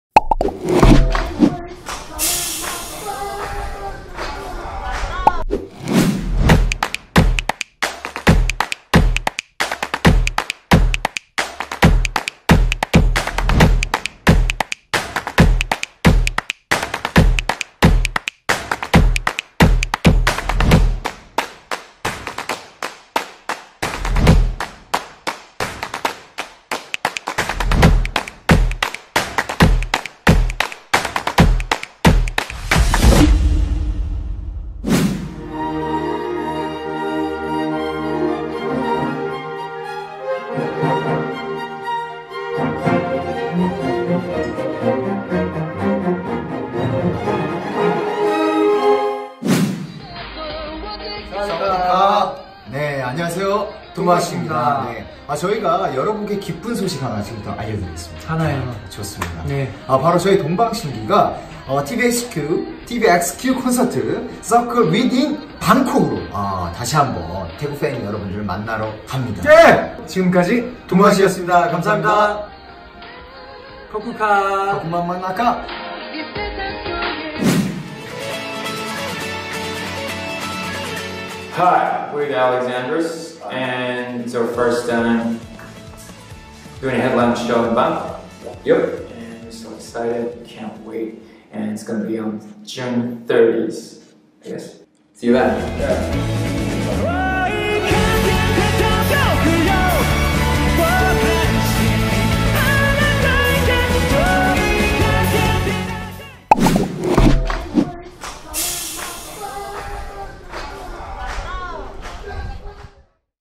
upcoming ประจำสัปดาห์ l 27 Jun – 03 Jul 2019
สัปดาห์นี้ มีงานเด็ดๆ เพียบ เลือกที่ใช่ แล้วชวนเพื่อนไปเอนจอยกัน  เช็คคอนเสิร์ตและงานแสดงน่าไปประจำสัปดาห์กับเราได้ใน upcoming ประจำสัปดาห์ l 27 June – 03 July 2019

•(RBSO) การแสดงนานาชาติเฉลิมพระเกียรติ 2562 : Emperors of Music

•TVXQ! CONCERT -CIRCLE- with in BANGKOK

การประกวด MISS UNIVERSE THAILAND 2019 : PRELIMINARY

•การประกวด MISS UNIVERSE THAILAND 2019 : รอบตัดสิน